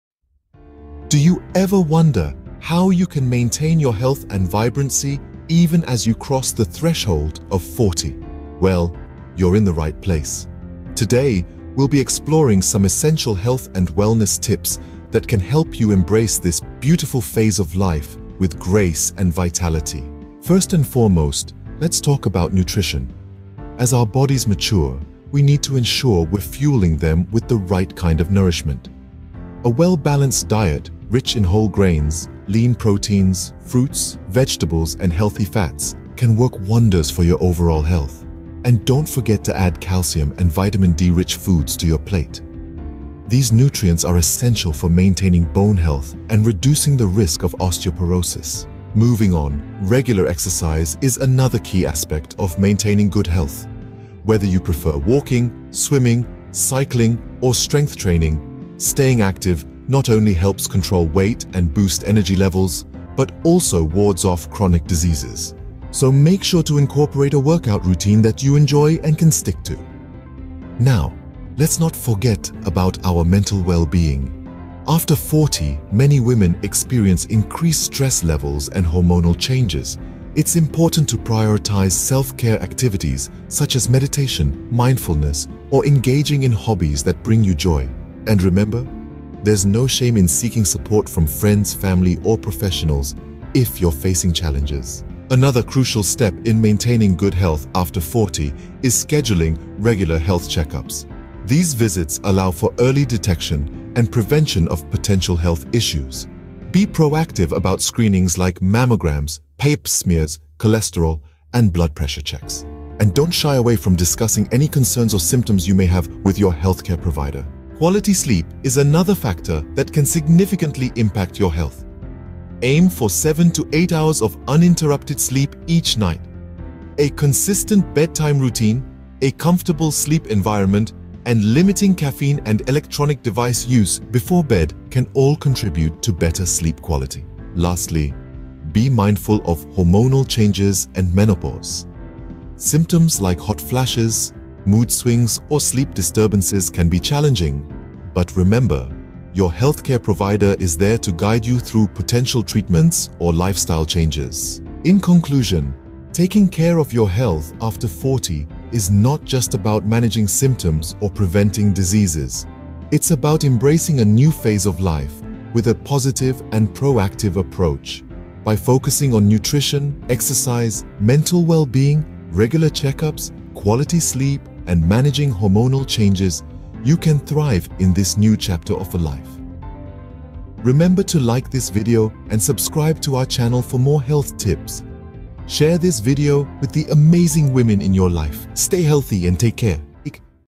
Thriving After 40: Essential Health Tips 2023
In this motivational video, we're diving deep into the keys to not just surviving but THRIVING in your 40s and beyond! 💪✨

🍏 Discover essential health tips to boost your physical and mental well-being.
🏋️‍♀️ Embrace a holistic approach to fitness, because age is just a number!
🧘‍♂️ Learn mindfulness techniques to navigate the challenges and joys of this vibrant stage of life.

🌈 It's never too late to prioritize your health and happiness! Join us on this journey to unlock your full potential. 🚀

🔑 Key Topics:

Nutrition for vitality 🥗
Fitness routines for all levels 🏃‍♂️
Mental well-being strategies 🧠
Embracing change and resilience 🌱